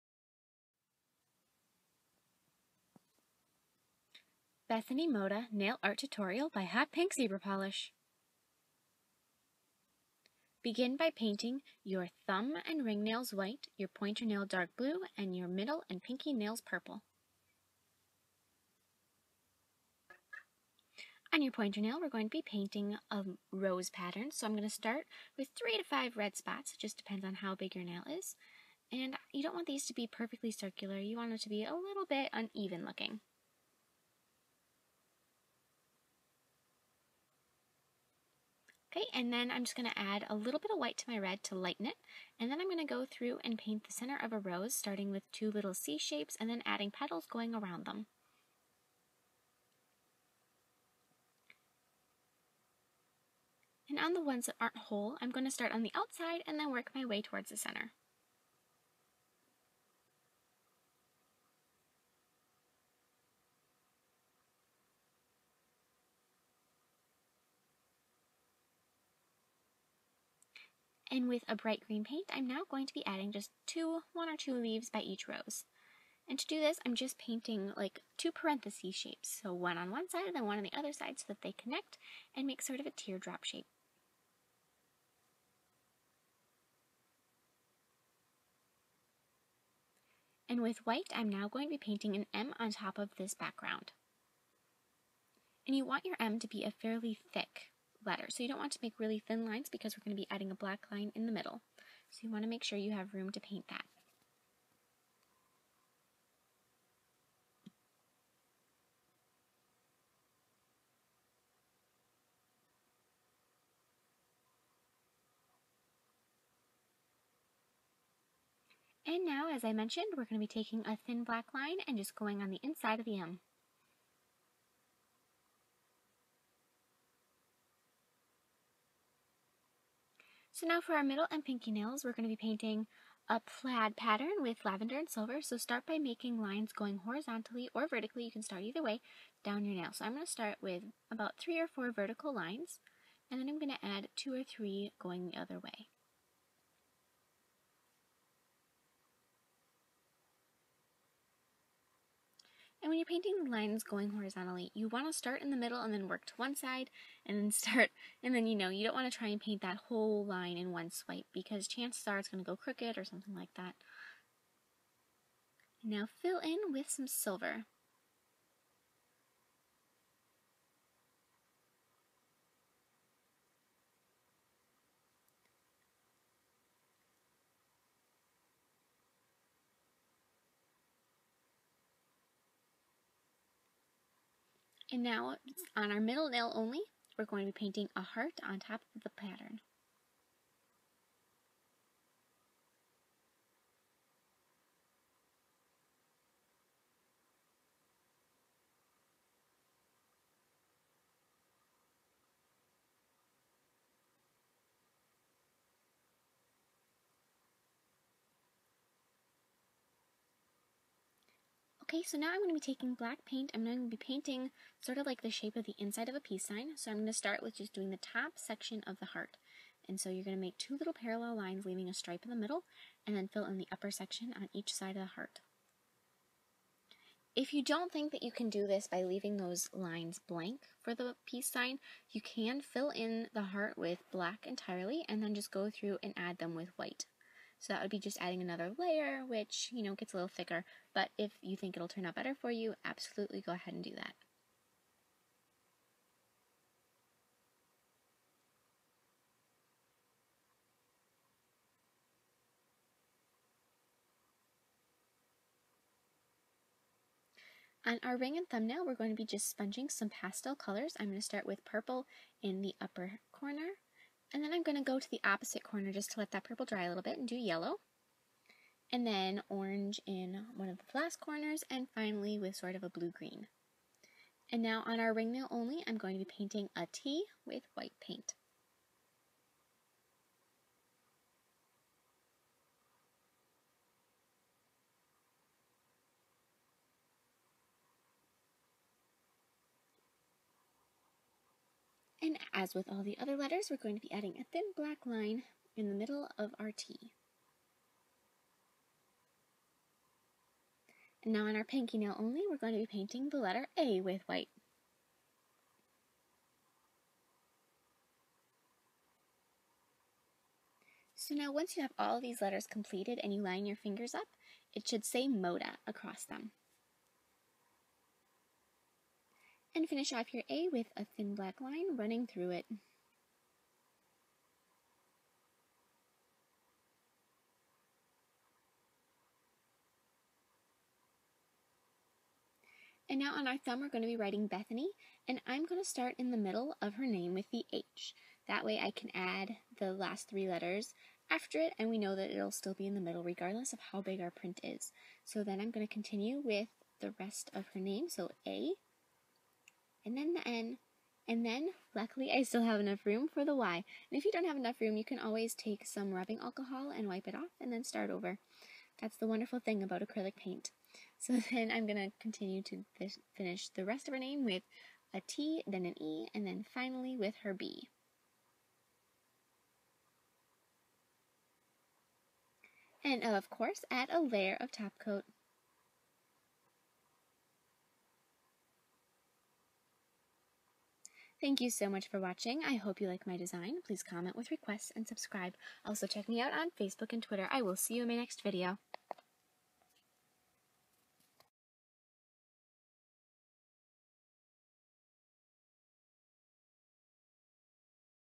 Bethany Mota Nail Art Tutorial (REQUEST)
This is a really cute design, even if you don't put the letters over the patterns! I hope you like!  Please share your recreations with me on facebook or twitter!